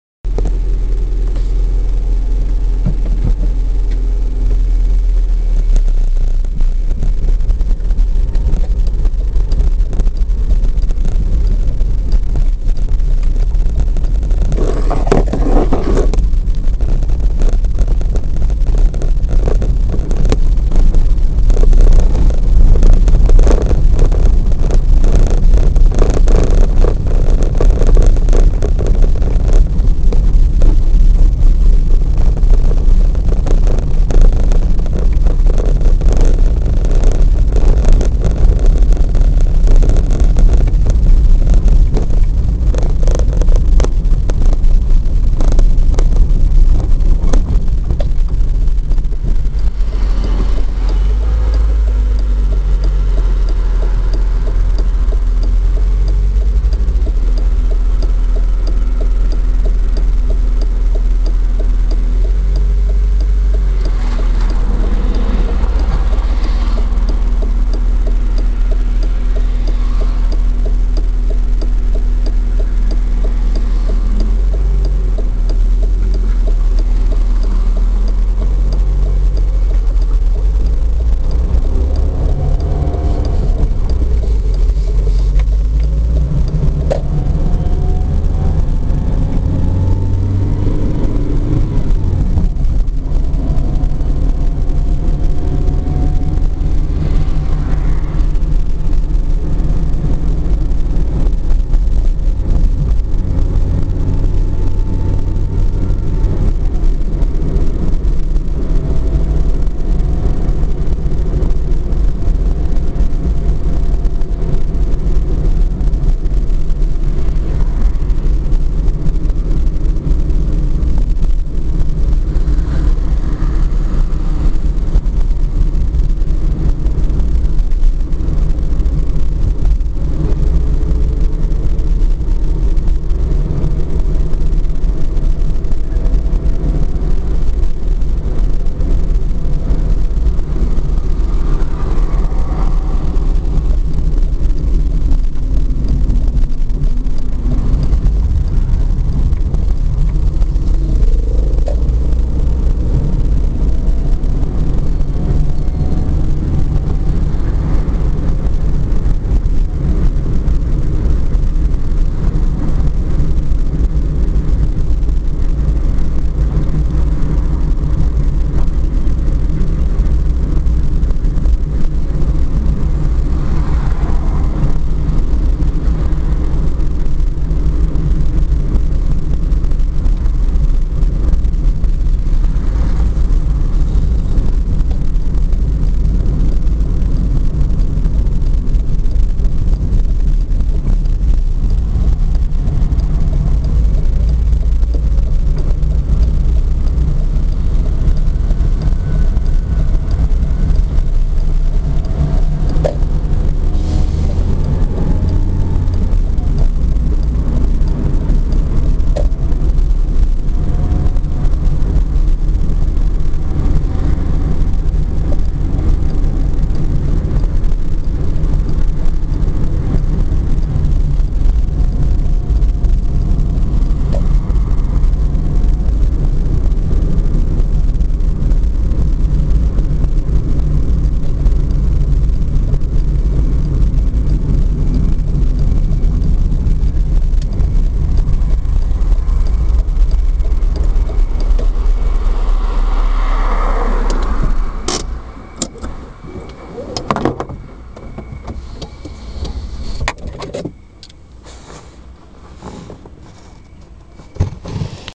Andando em Santo Antonio da Patrulha - Parte 1
Filmagem: 03/05/2012 09:04 - De Rua Isidoro Barcelos até Av. Afonso Porto Emerim - Santo Antonio da Patrulha - RS